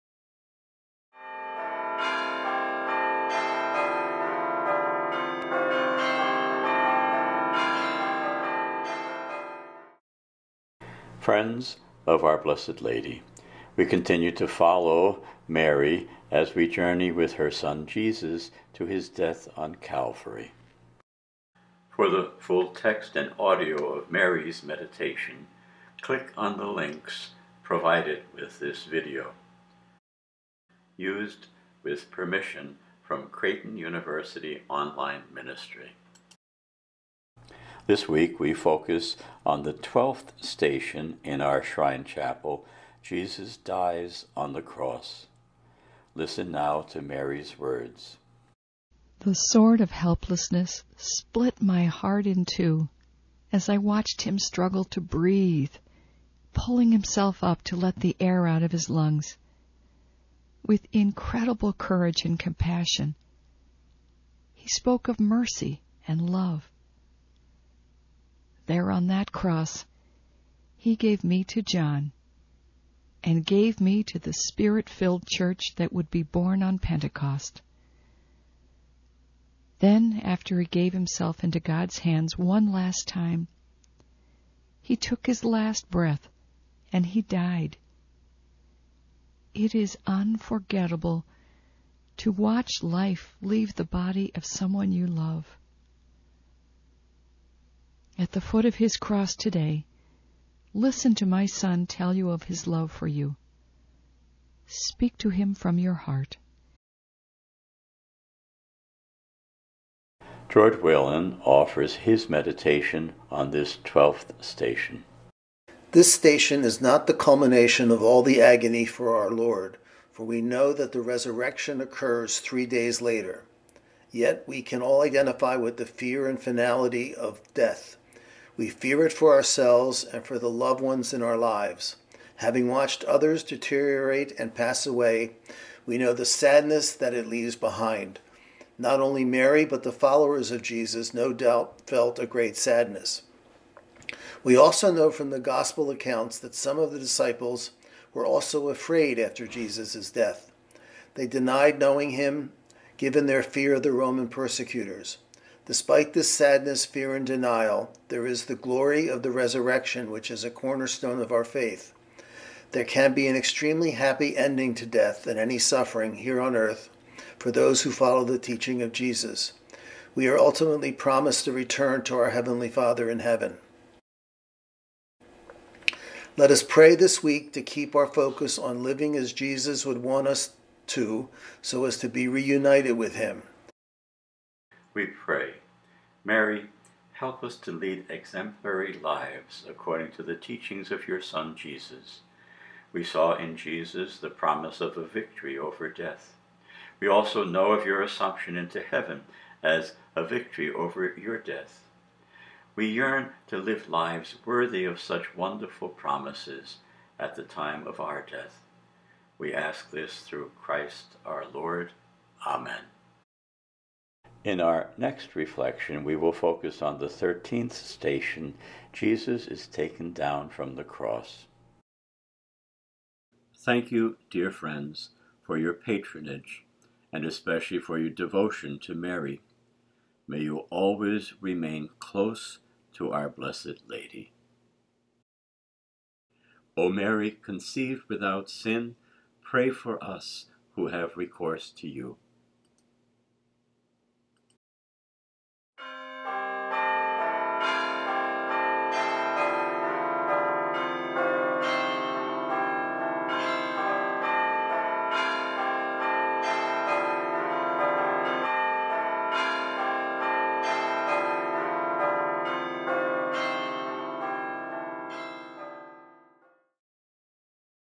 The Stations of the Cross - Twelfth Station: Jesus Dies on the Cross
Our Miraculous Medal Shrine houses a gorgeous set of the Stations of the Cross. They were created by a Parisian artist who used oil paint on top of copper plates. We believe this is the only such set that he created, since he died soon after finishing.

This week, we focus on the Twelfth Station of the Cross: Jesus Dies on the Cross.

𝑊𝑒 𝑎𝑟𝑒 𝑖𝑛𝑑𝑒𝑏𝑡𝑒𝑑 𝑡𝑜 𝐶𝑟𝑒𝑖𝑔ℎ𝑡𝑜𝑛 𝑈𝑛𝑖𝑣𝑒𝑟𝑠𝑖𝑡𝑦 𝑓𝑜𝑟 𝑡ℎ𝑒𝑖𝑟 𝑝𝑒𝑟𝑚𝑖𝑠𝑠𝑖𝑜𝑛 𝑡𝑜 𝑢𝑠𝑒 𝑡ℎ𝑒𝑖𝑟 𝑡𝑒𝑥𝑡 𝑎𝑛𝑑 𝑚𝑜𝑣𝑖𝑛𝑔 𝑎𝑢𝑑𝑖𝑜 𝑜𝑓 𝑡ℎ𝑒 𝑆𝑡𝑎𝑡𝑖𝑜𝑛𝑠 𝑜𝑓 𝑡ℎ𝑒 𝐶𝑟𝑜𝑠𝑠 𝑤𝑖𝑡ℎ 𝑀𝑎𝑟𝑦 – 𝑇ℎ𝑒𝑛 𝑎𝑛𝑑 𝑁𝑜𝑤.